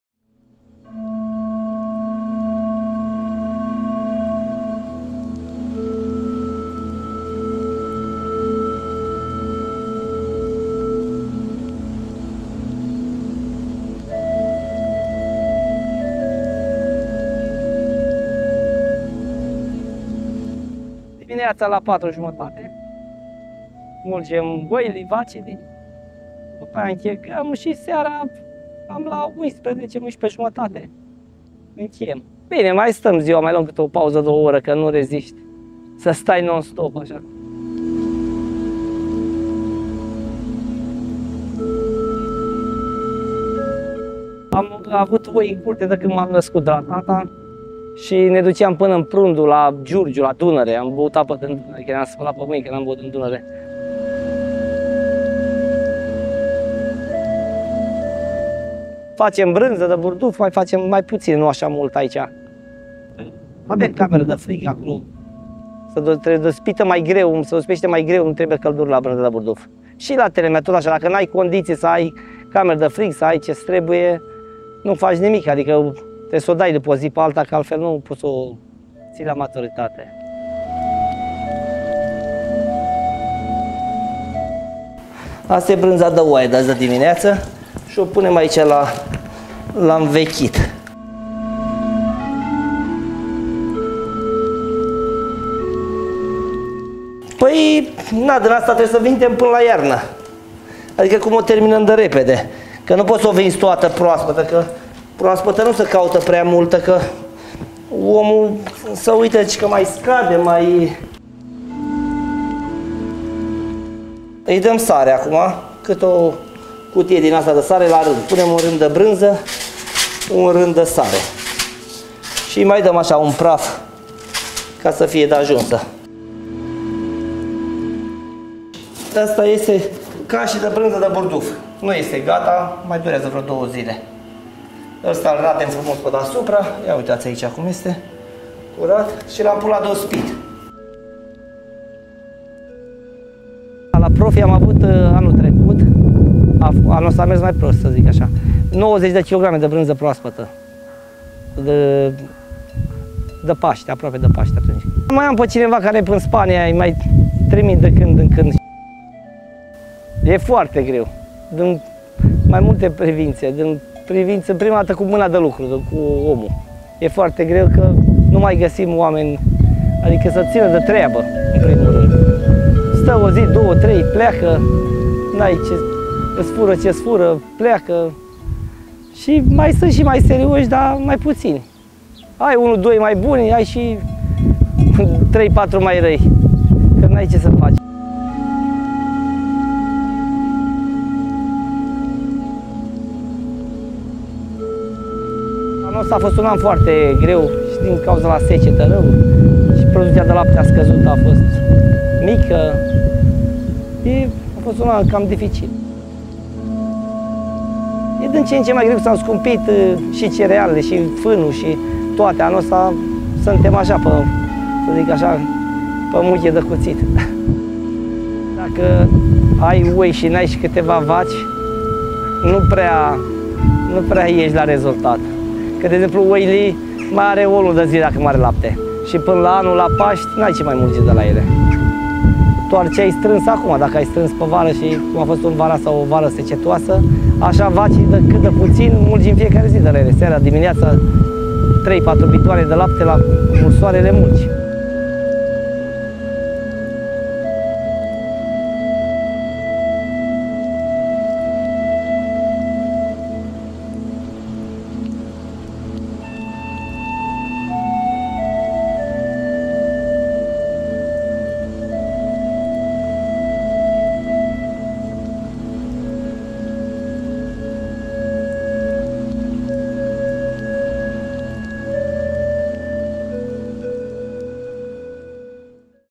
Află secretele din spatele de gustului delicios al branzeturi românești de la Cristinel Solomon
Jurnaliștii Gândul au plecat în explorarea României gustului autentic ghidați de Via Profi. Au poposit la poarta lui Cristinel Solomon pentru a vedea cum se pregătesc brânzeturile locale din laptele văcuțelor crescute în libertate pentru a avea cea mai bună calitate.

Familia Solomon crește animale de trei generații, iar secretele îngrijirii lor și ale plămădirii brânzeturilor savuroase au fost transmise din tată în fiu. Domnul Cristinel are grijă acum să respecte rețetele din bătrâni pentru ca oamenii care gustă din telemeaua, brânza de burduf sau cașul pregătit de el să se bucure de aroma de altădată.

Urmărește și tu interviul pentru a vedea de câtă dedicare și pasiune este nevoie pentru a oferi bunătăți tradiționale de calitate celor ce ajung în comuna Ulmi din județul Dâmbovița cu poftă de gustul bucatelor românești. Și nu uita să intri pe platforma de explorare culinară Via Profi pentru a descoperi sute de artizani din diferite colțuri ale țării care te așteaptă să le treci pragul.